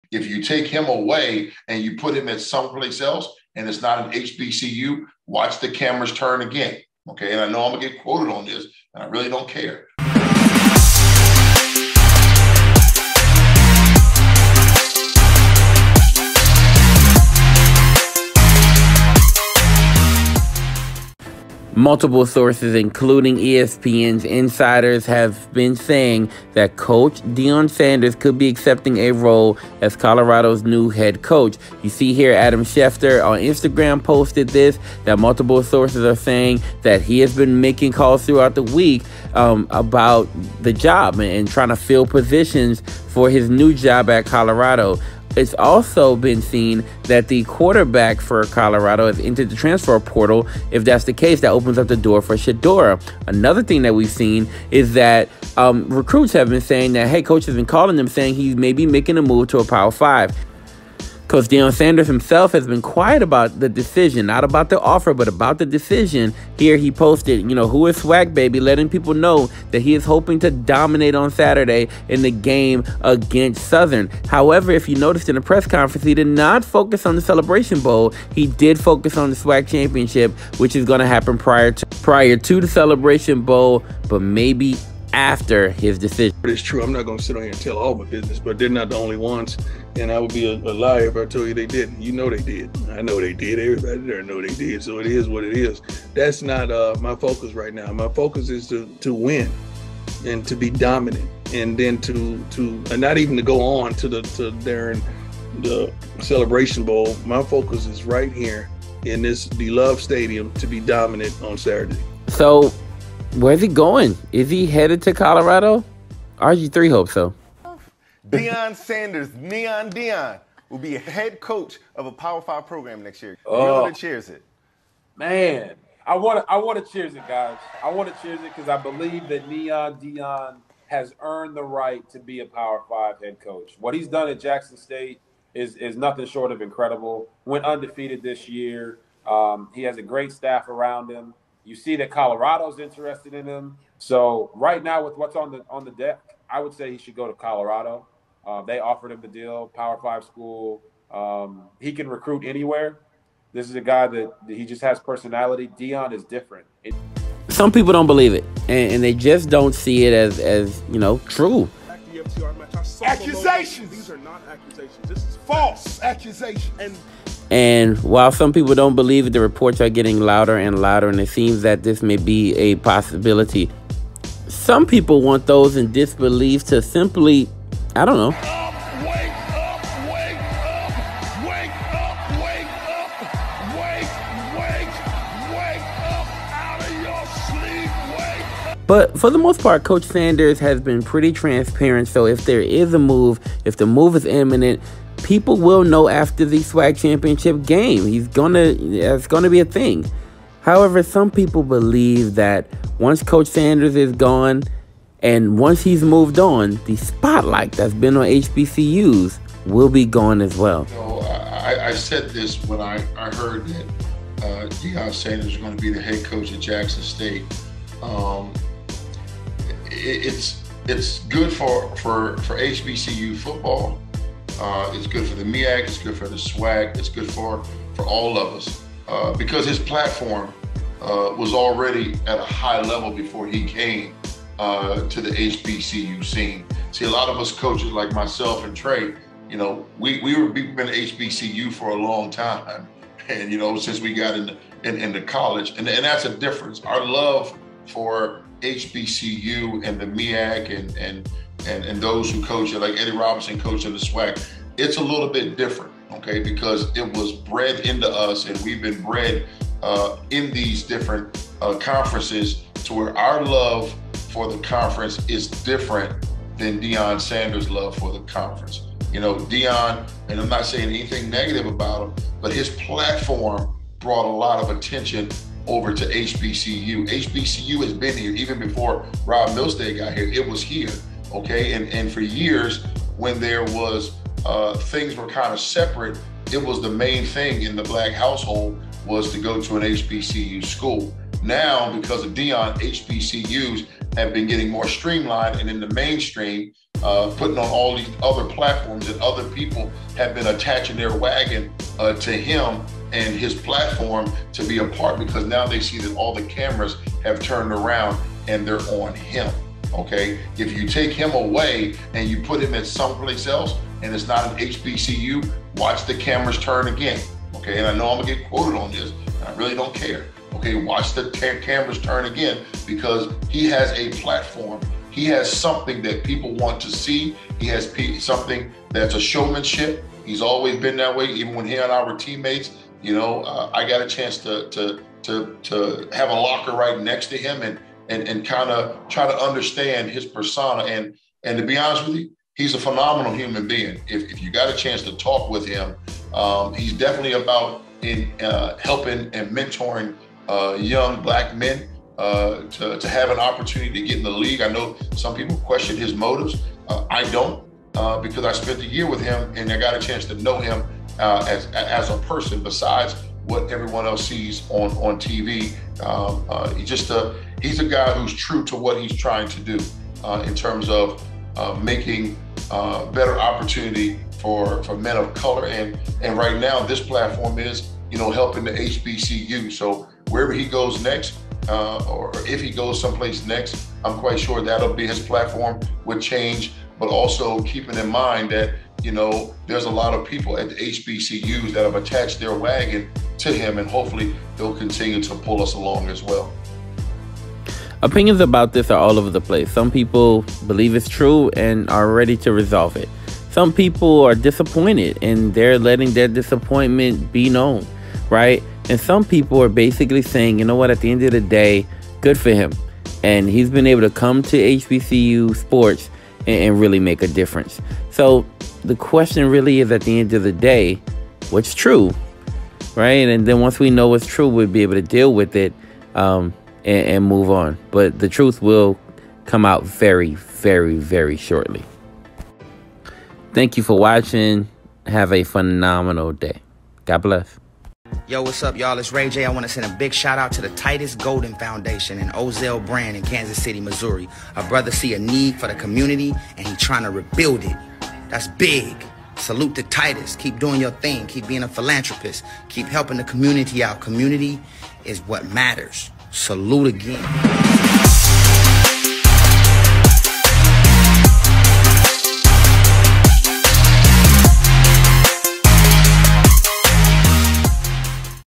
ESPN’s Peter Thamel reports Deion Sanders is taking the job in Colorado. Is it true?? Impact??
Is Coach Deion Sanders really leaving Jackson State after the SWAC championship game to Coach at Colorado? The evidence all points in that direction. What type of impact will the move have on HBCU football and Jackson State?